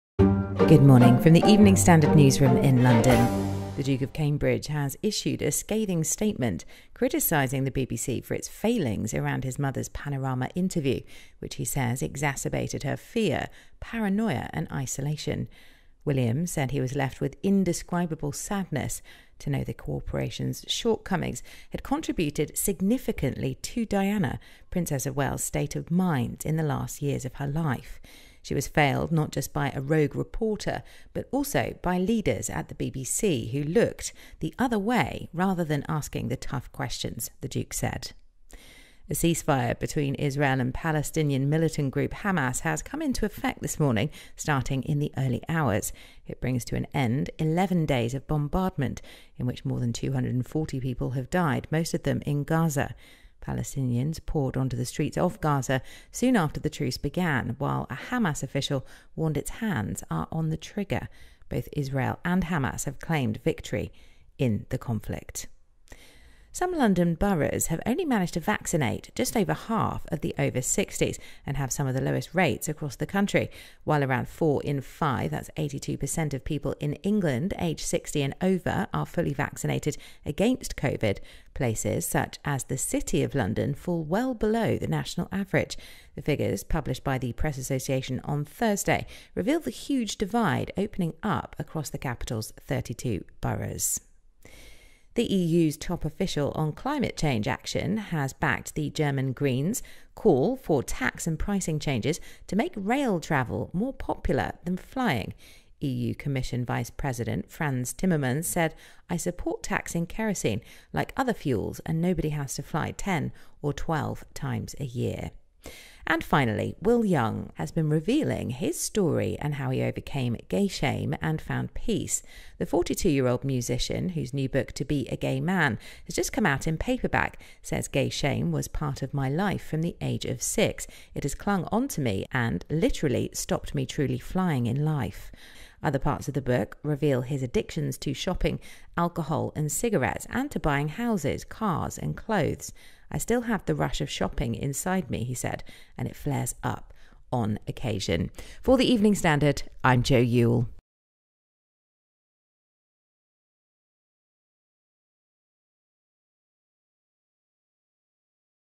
Making Headlines: Royal family condemn BBC's handing of Diana interview; plus Celebrations in Gaza
The Duke of Cambridge has issued a scathing statement criticising the BBC for its failings around his mother’s Panorama interview, which he says exacerbated her “fear, paranoia and isolation”.
William said he was left with “indescribable sadness” to know the corporation’s shortcomings had “contributed significantly” to Diana, Princess of Wales’ state of mind in the final years of her life.
“She was failed not just by a rogue reporter, but by leaders at the BBC who looked the other way rather than asking the tough questions,” the duke said.

A ceasefire between Israel and the Palestinian militant group Hamas has come into effect this morning, starting in the early hours.
It brings to an end 11 days of bombardment in which more than 240 people have died, most of them in Gaza.
Palestinians poured onto the streets of Gaza soon after the truce began, while a Hamas official warned its hands "are on the trigger".
Both Israel and Hamas have claimed victory in the conflict.

Some London boroughs have only managed to fully vaccinate just over half of over-60s and have some of the lowest rates across the country.
While around four in five (82 per) people in England aged 60 and over are fully vaccinated against Covid, places such as the City of London (54.5 per cent) fall well below the national average.
The figures, published by the Press Association on Thursday, reveal the huge divide opening up across the capital’s 32 boroughs.

The EU's top official on climate action has backed the German Greens' call for tax and pricing changes to make rail travel more popular than flying.
EU Commission Vice-President Frans Timmermans said "I support taxing kerosene like other fuels" and "nobody has to fly 10 or 12 times a year".

and finally.
Will Young has been revealing his story and how he overcame gay shame and found peace.
The 42 year old musician, whose new book To Be a Gay Man, has just come out in paperback, says “gay shame was a part of my life from the age of six. It has clung on to me and literally stopped me truly flying in life”.
After Pop Idol, 22 year Young publicly “came out” — an expression he uses but hates — to pre-empt a newspaper threatening to do the job for him. He revealed, that it’s hard today to comprehend how distressing that must have been, but admits in those days to being a gay pop star was asking to be abused; even his publicists urged caution.
“I wanted to be true to myself, yet I also wanted to have a long and successful career,” he says. Other parts of the book reveal his addictions — to shopping, alcohol and cigarettes and to buying houses, cars and clothes. “I still have the rush of shopping inside me, and it flares up on occasion.”